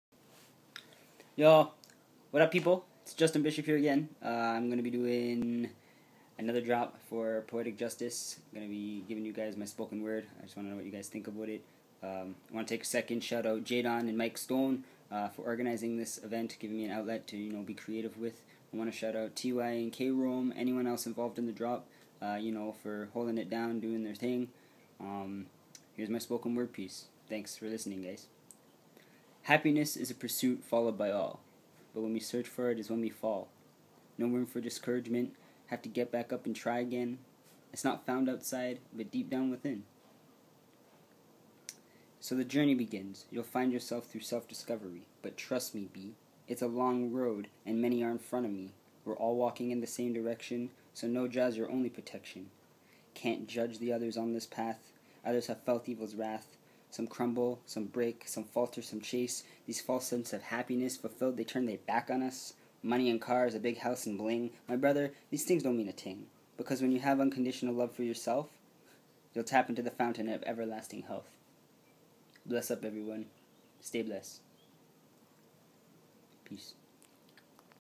Justin Bishop - pursuit of happiness Poetic Justice submis
A piece I wrote about our pursuit of happiness.